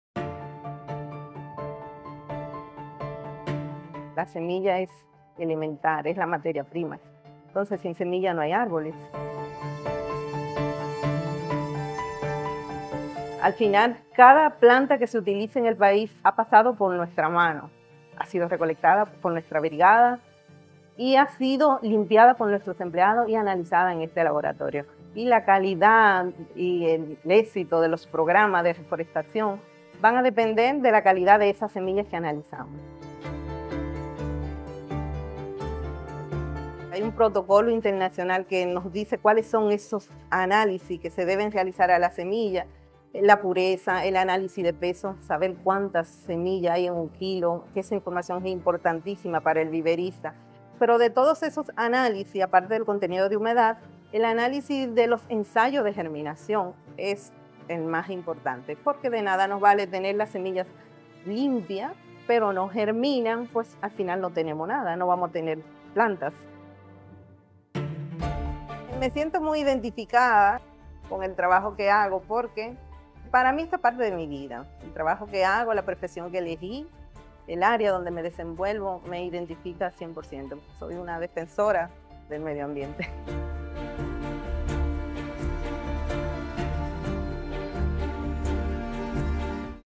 En nuestro Banco de Semillas, cada árbol que florece en nuestro país tiene su origen
Aquí, un equipo apasionado y comprometido asegura la calidad de cada semilla que luego se convertirá en vida para nuestras montañas y bosques. Su rol es vital: desde la recolección hasta los análisis de pureza y germinación, el trabajo que realizan es la base de nuestros programas de reforestación. Ellos son defensores del medio ambiente en cada etapa. 🌍💚

¡Únete a nuestro compromiso y planta un árbol!